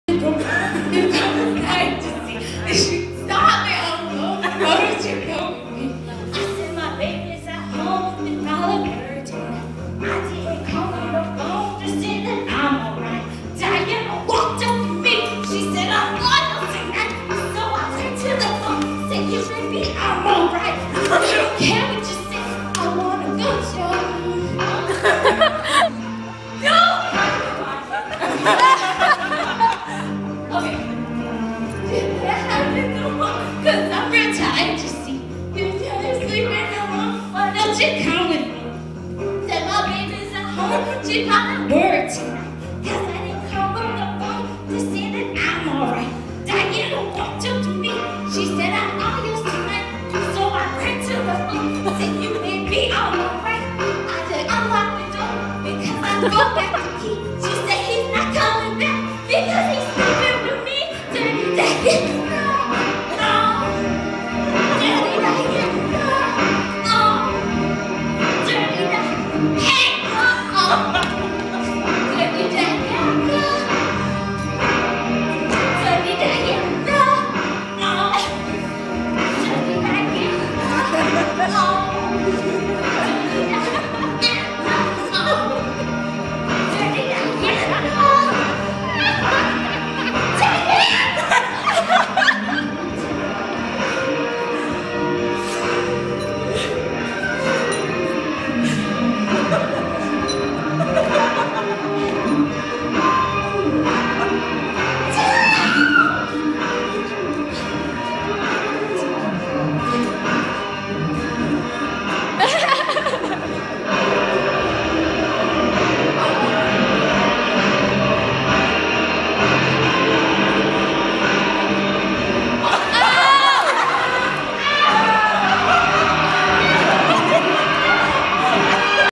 Jessica falls on her bummm
Jessica falls on her bum at karaoke night